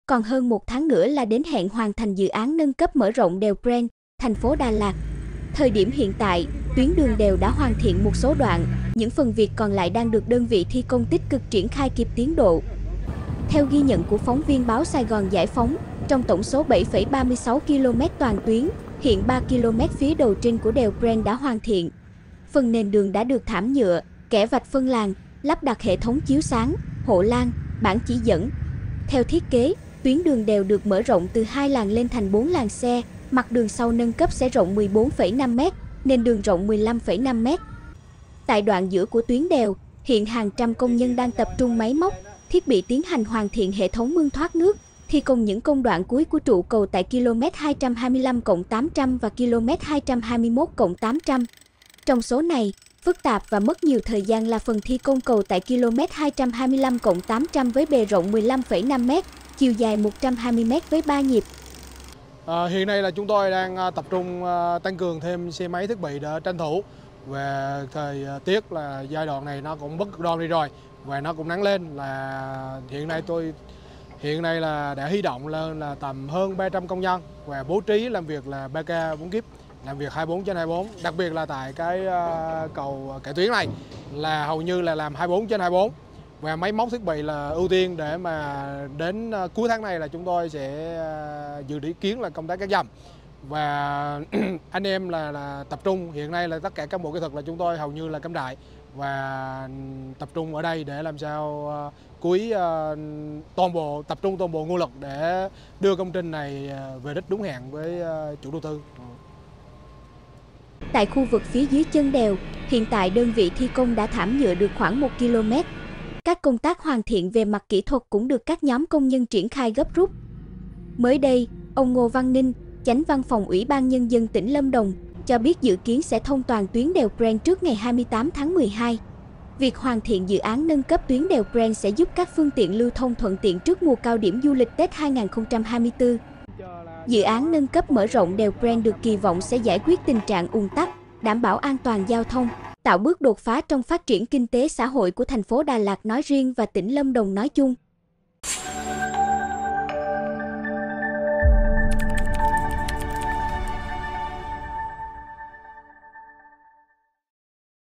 Đèo Prenn – Đà Lạt: Những ngày cuối nâng cấp, mở rộng
Còn hơn một tháng nữa là đến hẹn hoàn thành dự án nâng cấp mở rộng đèo Prenn, TP Đà Lạt. Thời điểm hiện tại, tuyến đường đèo đã hoàn thiện một số đoạn, những phần việc còn lại đang được đơn vị thi công tích cực triển khai kịp tiến độ.